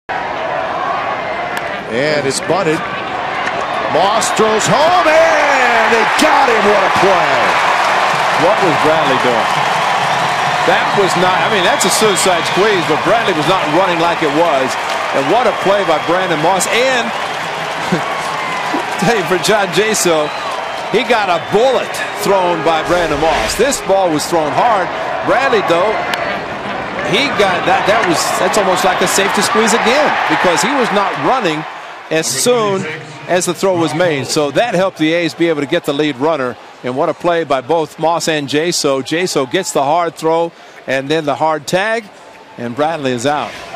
2013/07/13 Moss gets the out at home
BOS@OAK: Moss makes a quick throw home to save a run